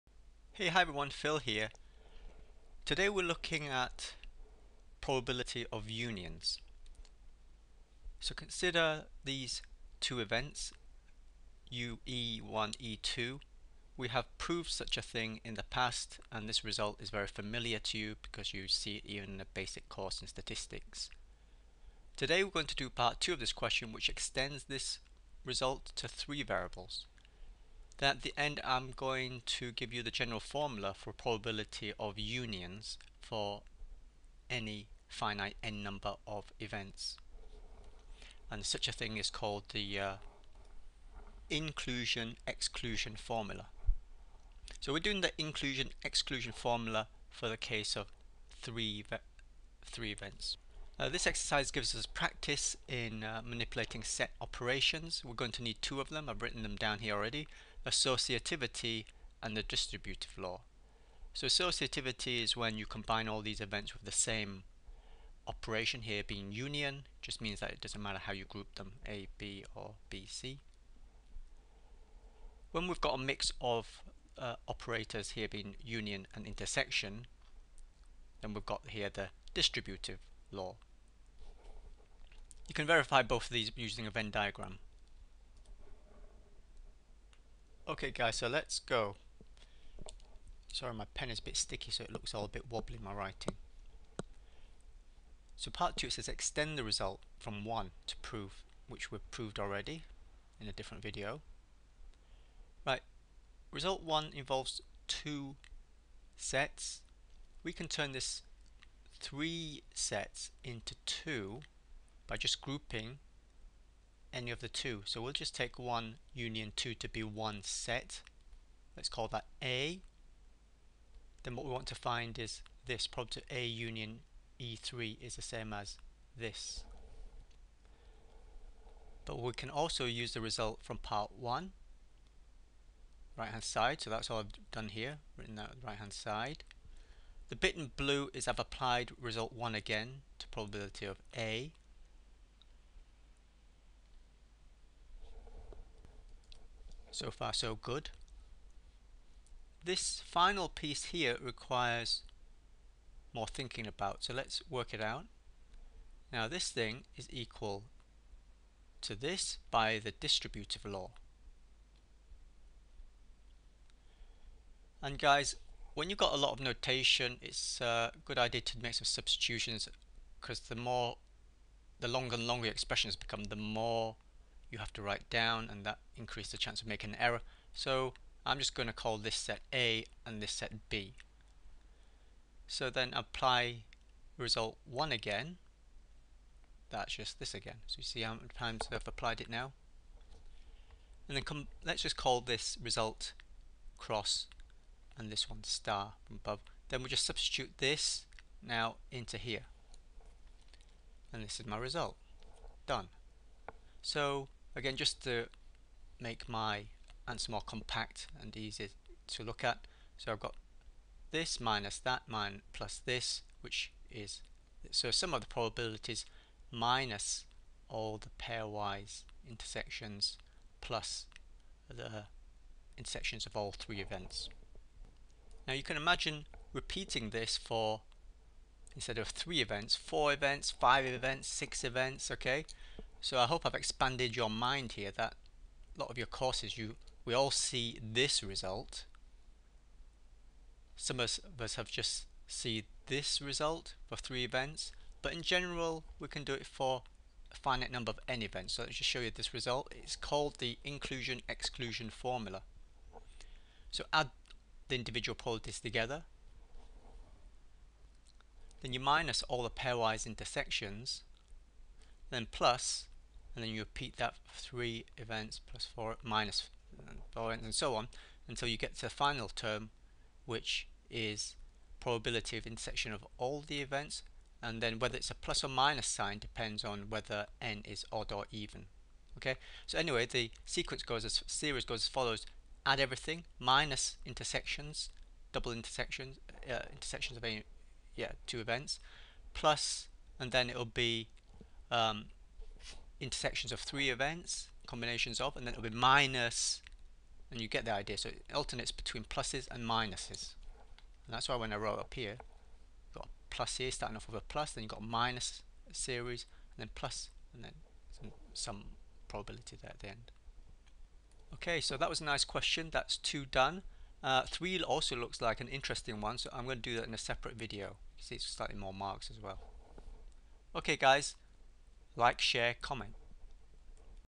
#5 Inclusion-exclusion formula for 3 events
Derivation of the inclusion-exclusion formula for 3 events.
Statistics worked examples